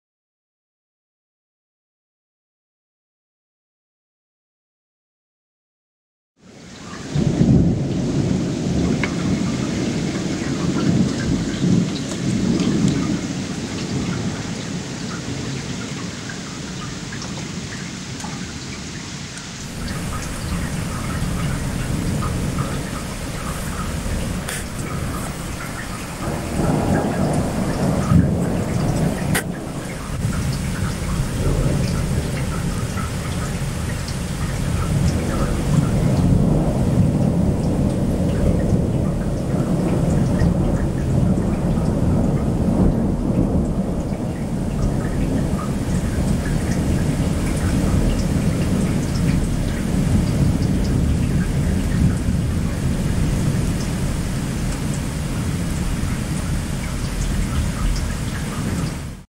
Gewitter Fuldaaue - Energie eines Gewitters
Habe 2010 ein Gewitter gefilmt, bei dem es fast nicht regnete, insofern ist das Bild etwas besser.
Alleine die Lichtenergie ist sehr beeindruckend, allerdings steckt der größte Energiebetrag natürlich in der elektrostatischen Aufladung der Blitze selbst.